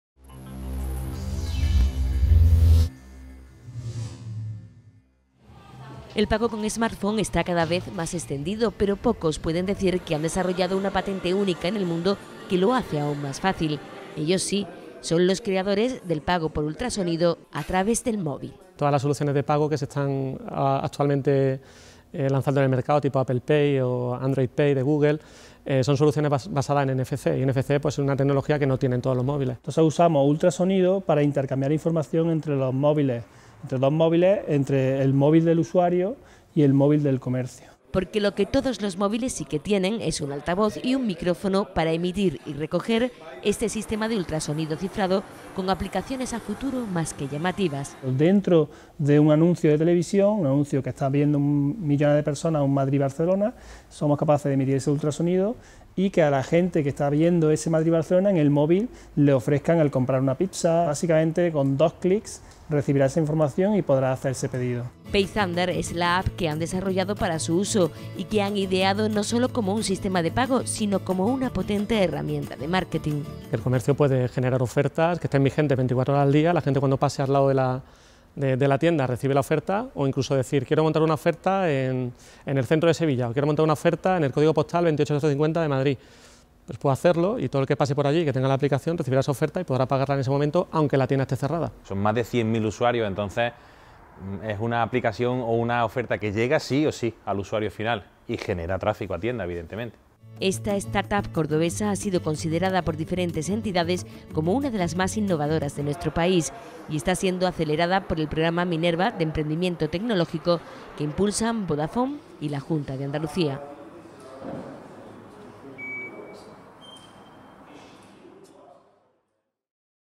HDL Una startup patenta un sistema de ultrasonido para pagos con móvil
Una startup de Córdoba ha patentado un sistema único en el mundo por el que permite el pago con móvil a través de un sistema de ultrasonido. Esta patente pretende extender este modo de pago a todos los móviles independientemente de la marca y el sistema operativo puesto que solo necesita un altavoz para emitir y un micrófono para recibir. PayThunder es la app que han desarrollado para su uso, que prevén hacer extensible a la televisión desde la que cualquier empresa a través de un anuncio podría enviar al móvil sus ofertas y el usuario hacer uso de ellas con un solo clic. PayThunder forma parte del Programa Minerva de emprendimiento tecnológico que impulsan Vodafone y la Junta de Andalucía.